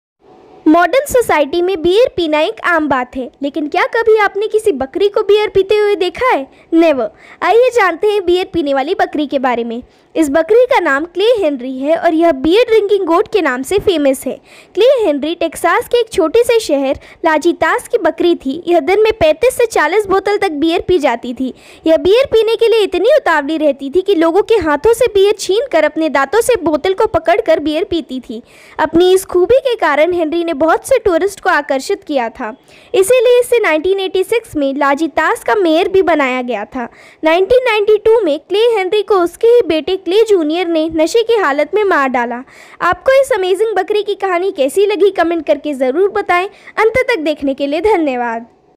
OMG!😱 क्या आपने Beer🍻 पीने वाली बकरी देखी है ?🤯🔥| Beer🍺 Drinking Goat🐐
Hey Guys !
Welcome back to Astromika Shorts.
Watch this video to know about a goat who can drink beer 🍾🍷